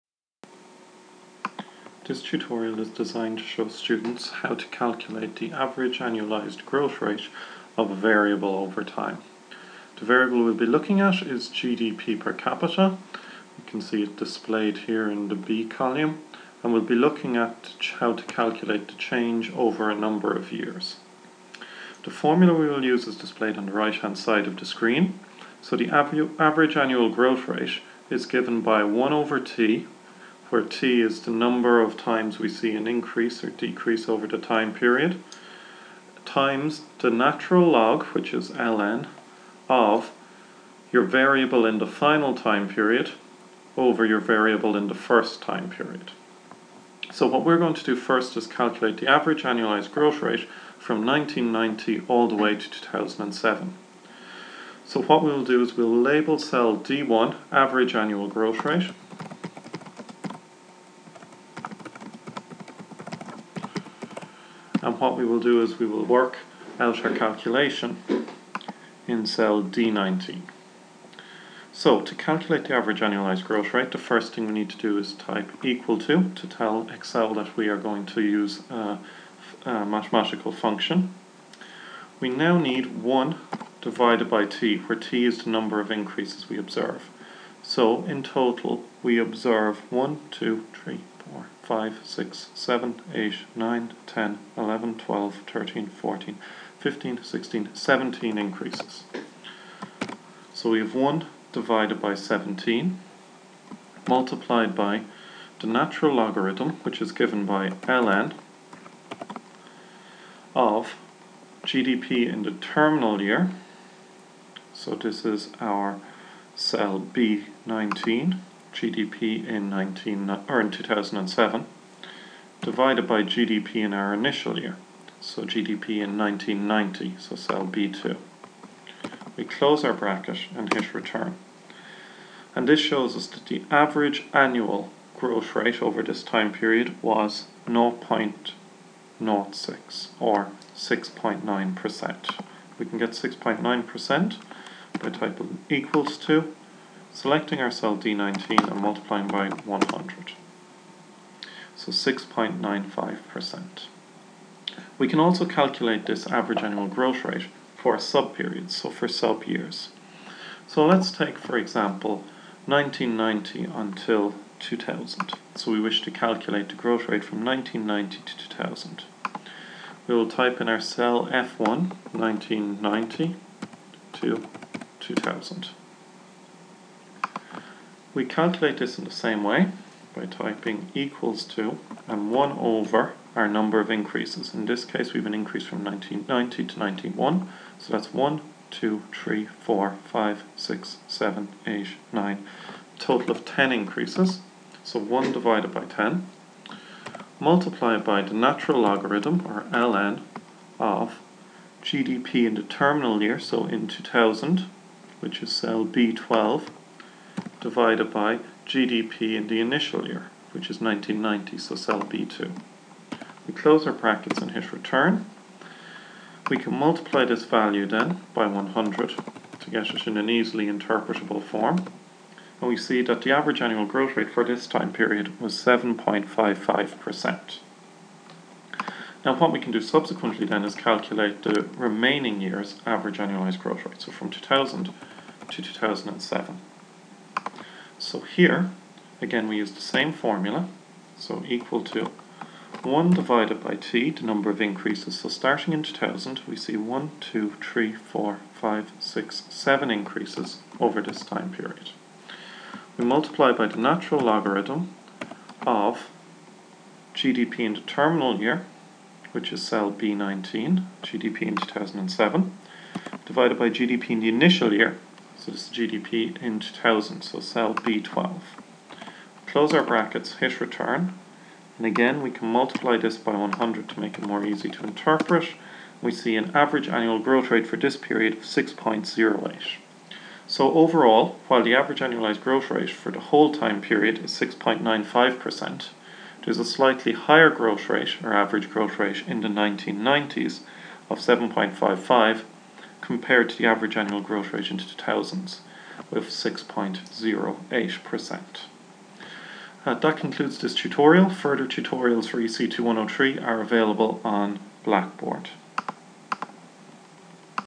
EC3115: Tutorial 3 - Calculating Average Annual Growth Rates
This video shows how to calculate the average annualized growth rate of a variable over time.  It is designed to help students in EC3115 to complete their assignment.